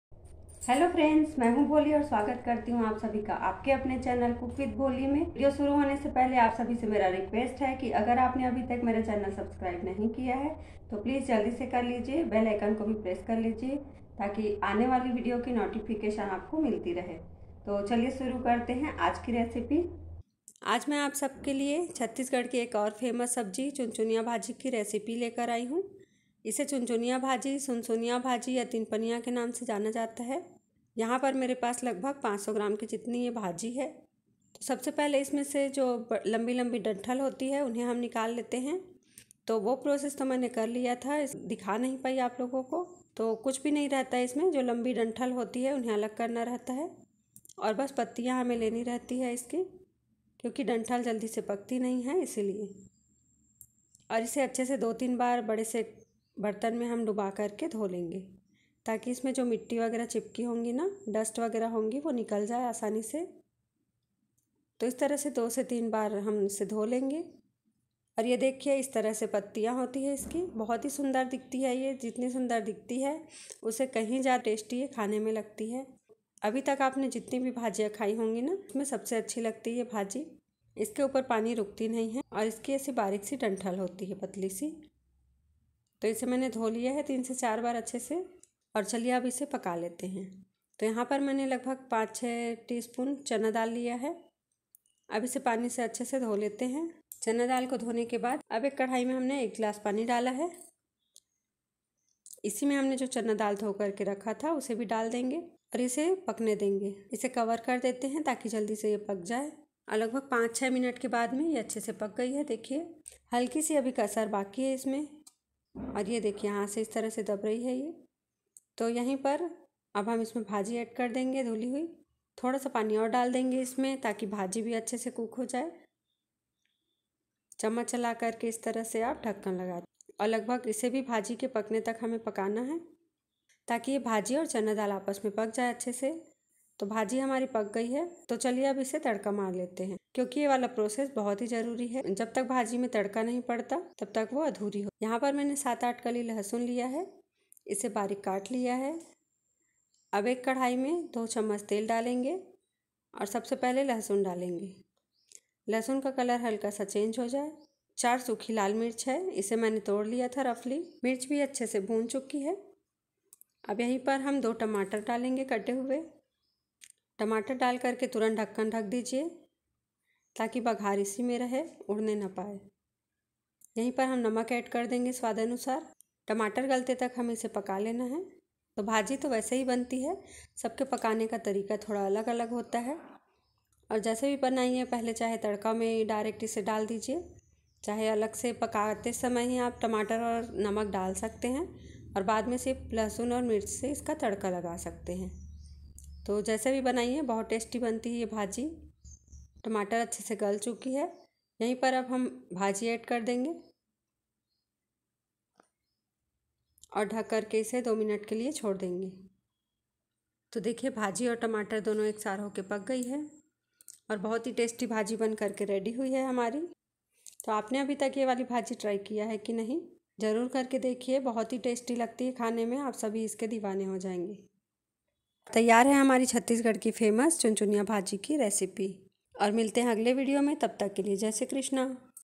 ||छत्तीसगढ़ की स्पेशल सुनसुनिया भाजी की रेसिपी| sunsuniya bhaji recipe|
hii friends aaj ki रेसिपी hai chhattisgarh ki prasiddh chunchuniya bhaji ki jo bahut jyada testy hoti hai our vitamins se bharpur hoti hai video pasand aye to like share our channel ko subscribe kijiye 🙏🙏🙏🙏